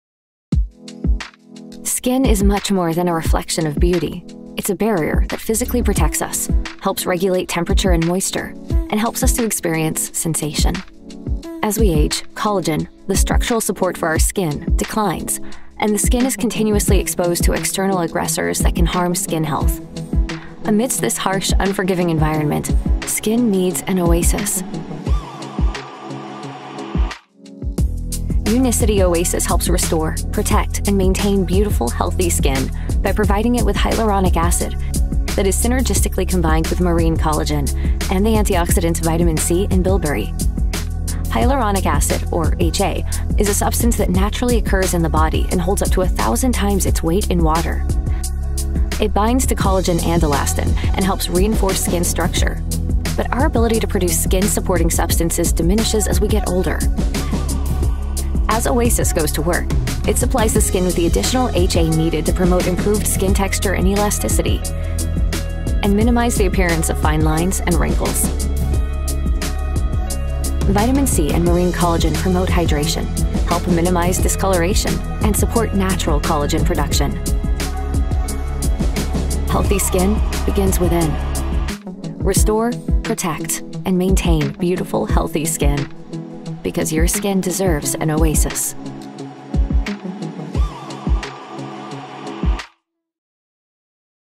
Unicity Oasis | An oasis for your skin
Unicity Oasis is a refreshing, berry-flavored beverage formulated with hyaluronic acid, vitamin C, and marine collagen to help maintain beautiful skin. Oasis is designed to hydrate thirsty skin, especially as we get older, and help protect it from external aggressors. It does all this from the inside out, because just as beauty comes from within, so does skin health.

Learn more about how hyaluronic acid promotes improved skin texture and minimizes the appearance of fine lines and wrinkles